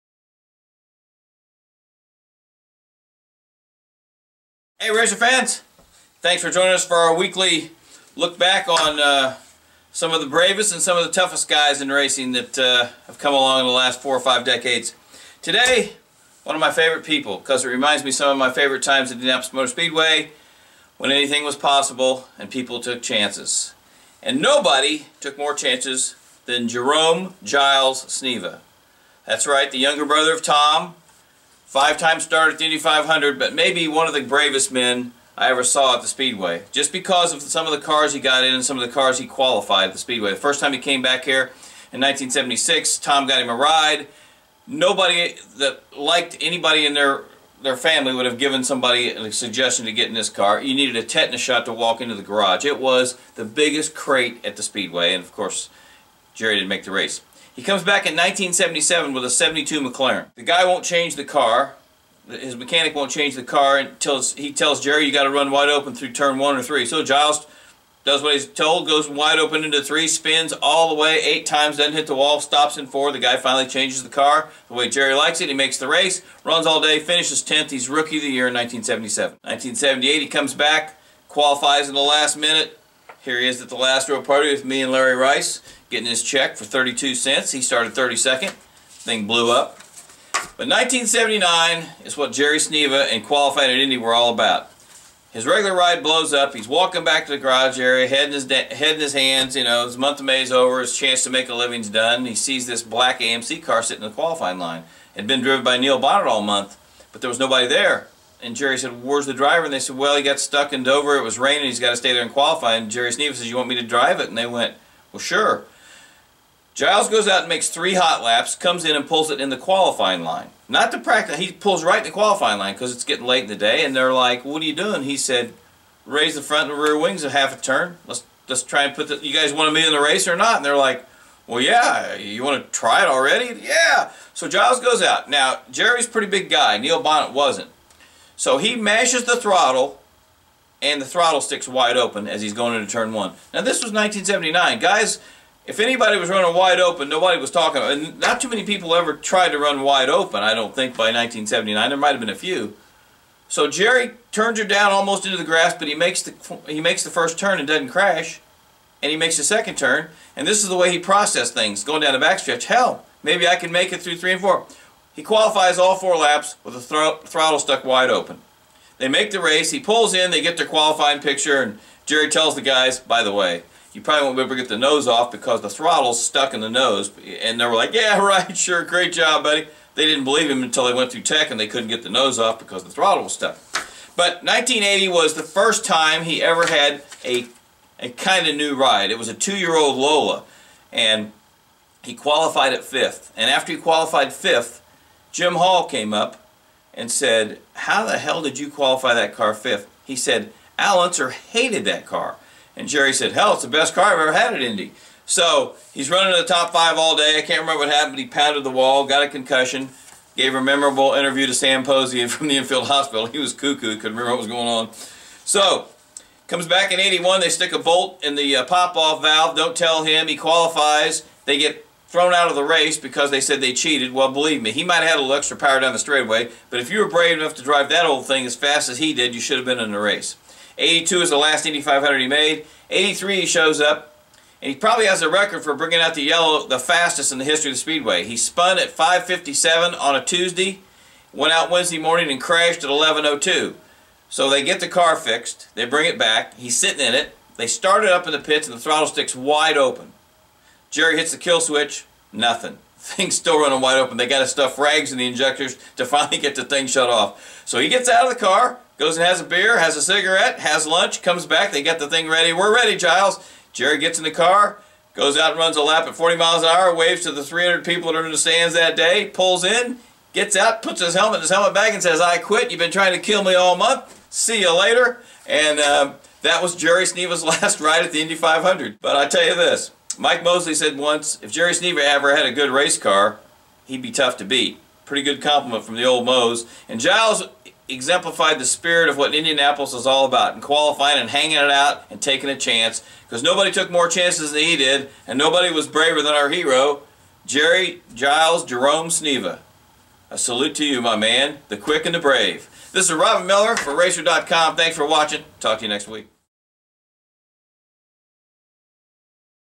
RACER: Robin Miller on Jerry Sneva
He may not have been the most accomplished member of his fast family but Jerry "Giles" Sneva made the most out of his opportunities at the Indianapolis Motor Speedway.
          In 1977, he qualified a five-year-old McLaren for his initial Indy 500, finished 10th and earned rookie-of-the-year honors. The next May he stuck an even older McLaren in the field with a banzai run in the closing minutes of time trials but he saved his real magic for 1979.
          After his assigned car had blown its fifth and final engine of the month, Sneva was walking down pit lane and saw a car with no driver. Informed that NASCAR's Neil Bonnett was stuck in Dover waiting out the weather, Giles offered his services. Following a couple practice laps he went out to qualify and was presented with a stuck throttle as he barreled into Turn 1. He didn't crash, adjusted his line and braved out four laps with a jammed throttle to make the starting lineup.
          In 1980, he qualified a two-year-old Lola fifth fastest, prompting a compliment from Jim Hall about his old car.
          His last Indy 500 start came in 1982 and, following a couple of spins and another stuck throttle during practice in 1983, Jerry called it a day.

          Brother Tom won Indy and a pair of national driving titles but Giles earned a lot of respect and five starts at Indy with talent and balls.